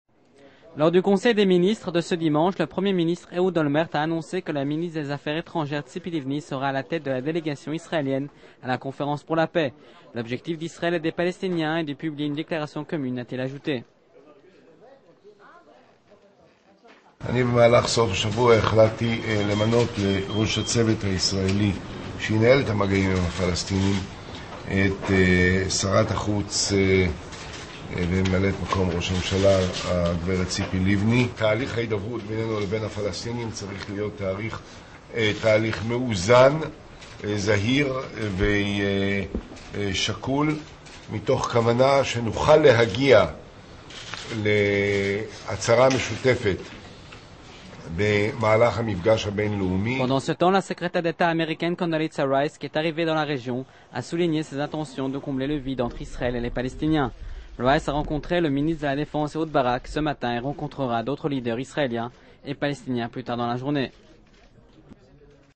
Nouvelle visite de Condoleeza Rice
Pendant que le Premier ministre Ehoud Olmert met en place les membres de la délégation israélienne qui s'envolera pour Annapolis en novembre prochain, la Secrétaire d'Etat américaine Condoleeza Rice est arrivée dans la région pour rencontrer les différents protagonistes en vue de préparer cette conférence pour la paix. 14/10/2007